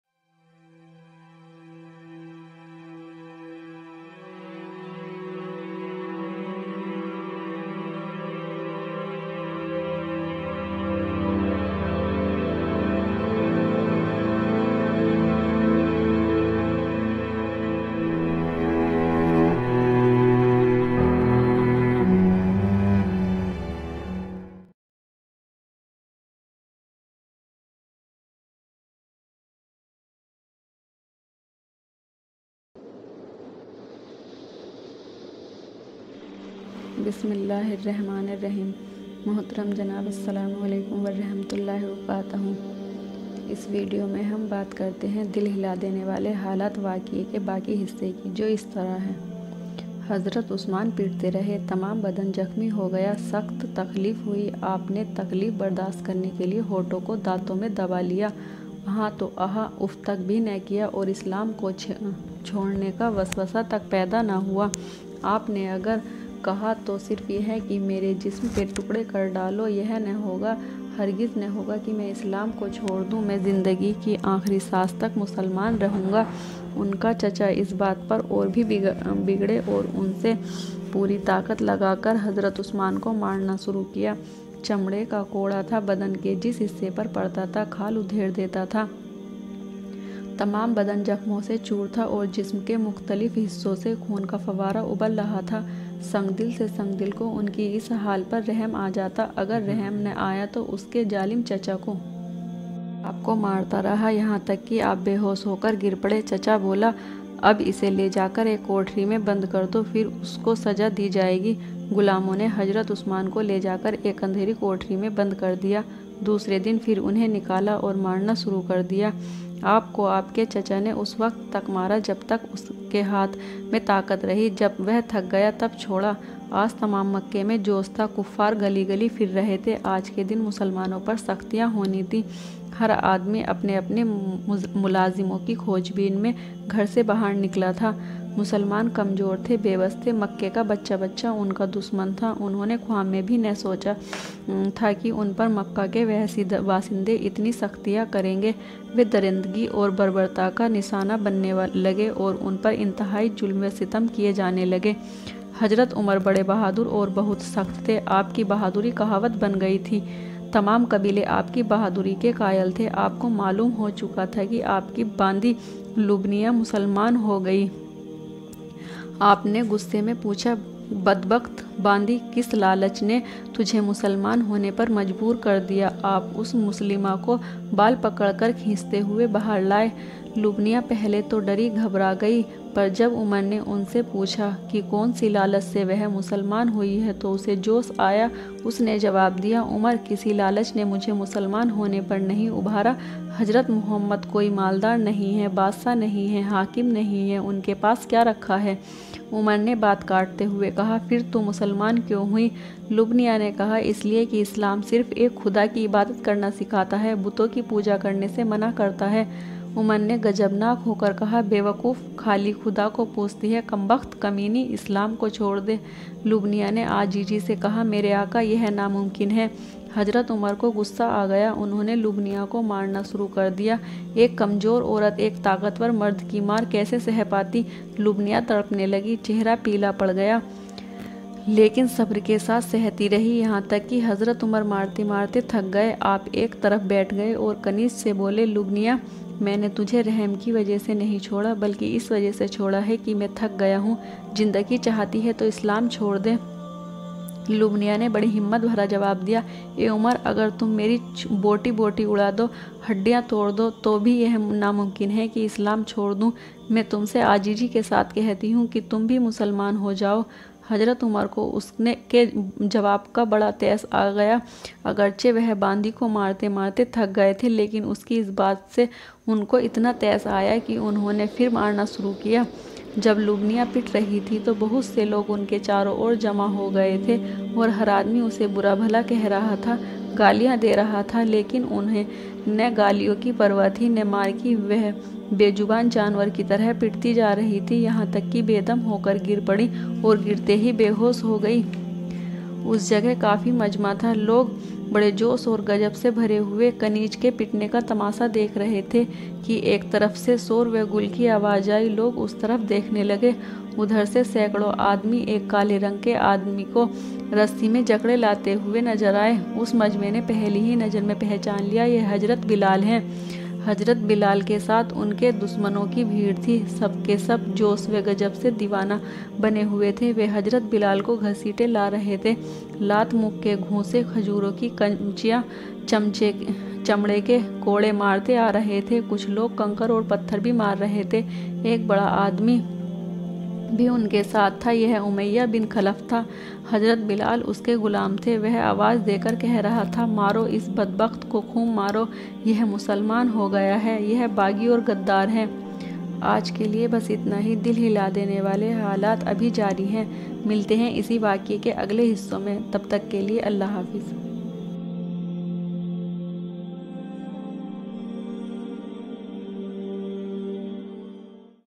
दिल हिला देने वाले हालत 2 | Episode 12 (B) || Aaftabe Alam Audio Book [HINDI]
इस वीडियो में बताया गया हर एक किस्सा हमारे प्यारे नबी हज़रात मुहम्मद सल्ललाहु अलैहि वस्सल्लम के बारे में है जो की आफताब ए आलम किताब मैं बताया गया है , ये किताब एक नावेल की तरह है जो हिंदी में लिखी हुई है , जिसके कुछ शब्द हिंदी भाषा में लिखे ( जो आम बोल चाल की भाषा में इस्तेमाल होते है ).

इस वीडियो में बताई गयी हर एक बात आफताब ए आलम किताब से ली गयी है , जिसमे अपने किसी भी बात का या शब्द का कोई इस्तेमाल नहीं किया गया ।।

Sunnat e Rasool Islamic Taqreer | Aaftabe Alam | Part -1 | Bismillah | Islamic Speech in Urdu/Hindi | Best Islamic Takreer

अल्लाह हम सबको सुनने की और समझने की तौफीक अता फरमाय । अमीन ॥

Source : Aaftaabe Alam Book (Novel) (Part 01)
Writer : Maulana Sadik Hussain Sardhanvi Marhoom

Editor : Arman
Voice : Gulista Shakeel